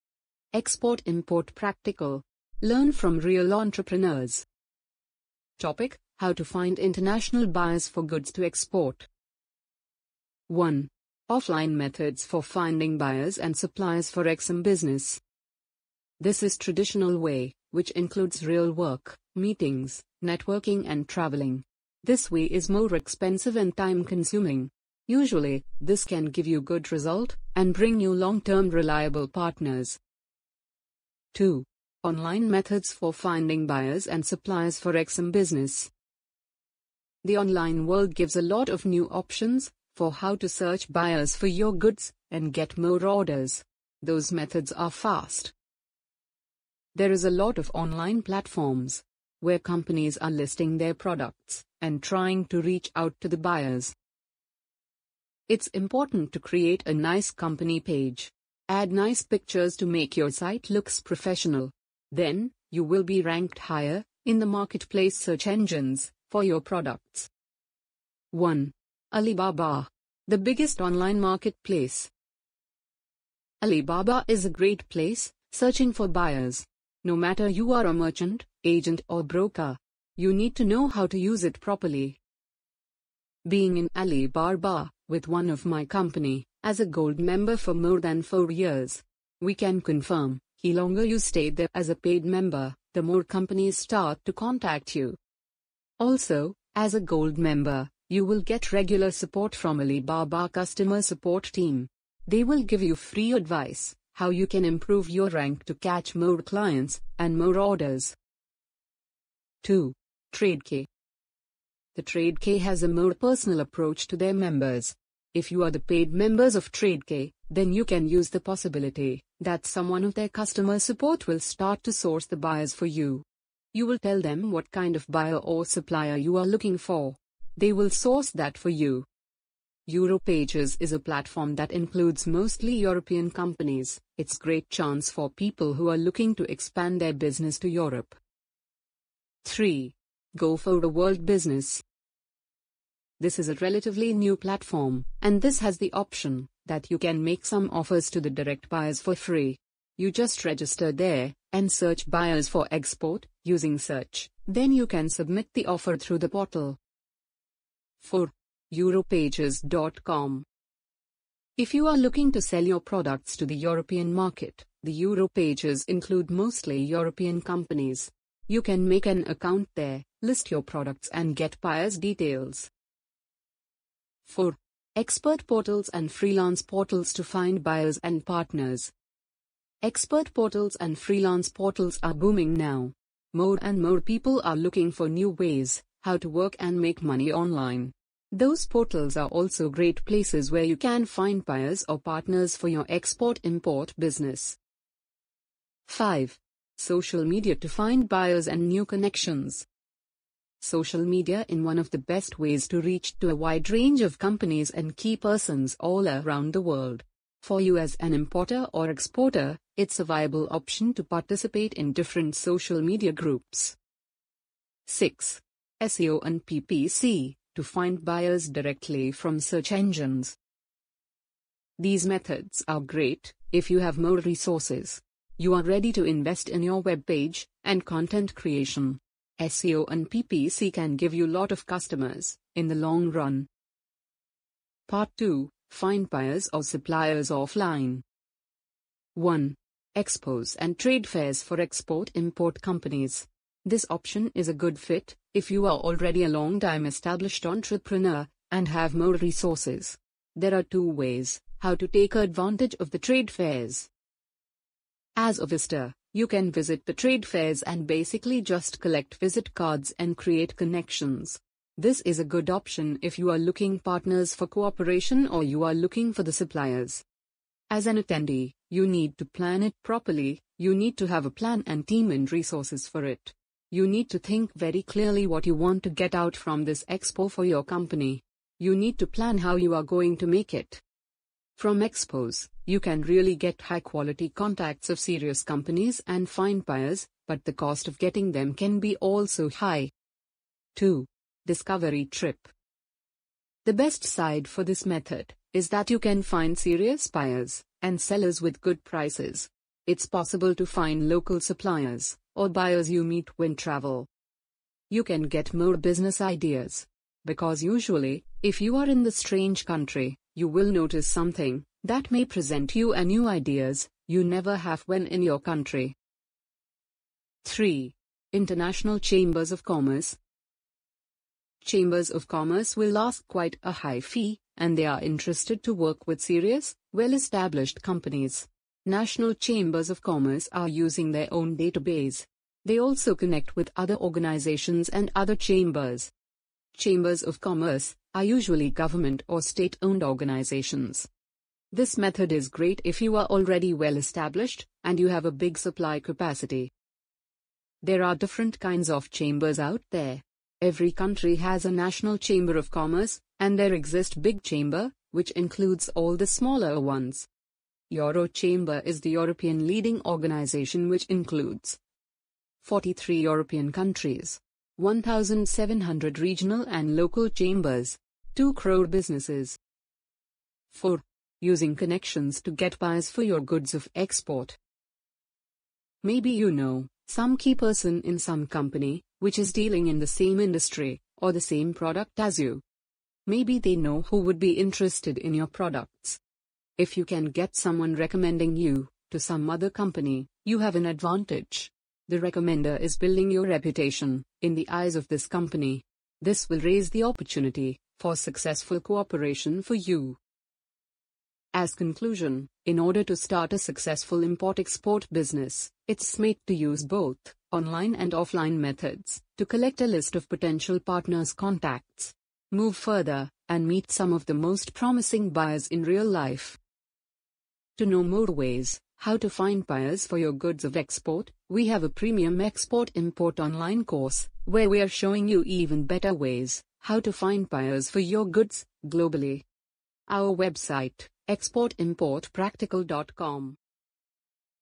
HOW TO FIND INTERNATIONAL BUYERS FOR GOODS TO EXPORT
In order to start a successful import-export business, it’s smart to use both, online and offline methods for searching and finding buyers. I would suggest to search for buyers online and collect a list of potential partners' contacts and then move further and meet some of the most promising ones in real life.

If you want to know more ways, how to find buyers for your goods of export, then we have a premium export-import online course, where we are showing you even better ways how to find buyers for your goods, globally! Enroll below: